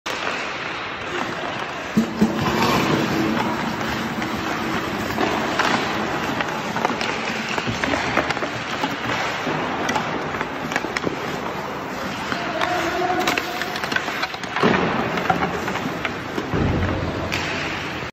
Stickhandling around everything but the kitchen sink NHLFanSkills | Hokejové zručnosti z Petržalky
🇸🇰Roman Kubik from Petrzalka, Slovakia has to dodge quite the barrage to keep stickhandling | NHLFanSkills Roman Kubík z bratislavskej Petržalky sa s pukom na hokejke dokázal vyhnúť všetkému, čím ho zasypali spoluhráči

Feeling social?